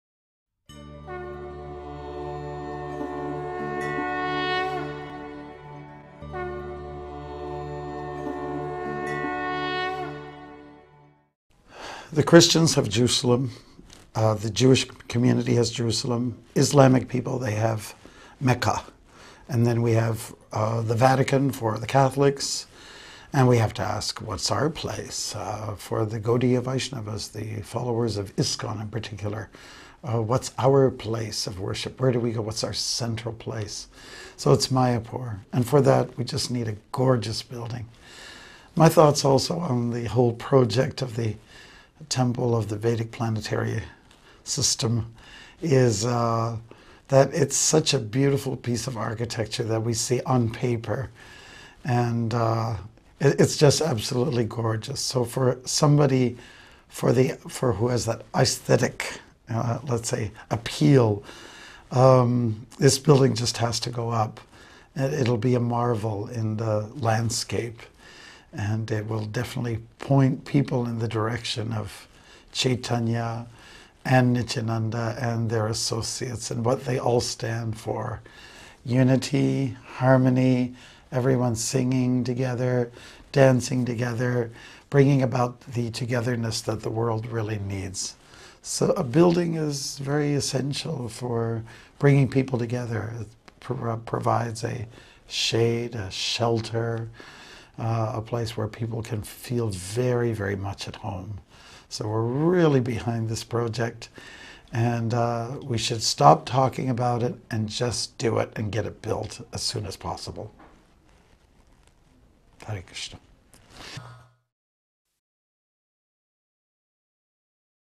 Temple of the Vedic Planetarium - His Holiness Bhaktimarga Swami
His Holiness Bhaktimarga Swami speaks about the need for an ISKCON/Gaudiya Vaishnava central place of worship like Mecca, Jerusalem and the Vatican, as well as the marvel and beauty of the TOVP and its ability to attract the world to Krishna Consciousness. "Stop talking about it and just get it built as soon as possible!"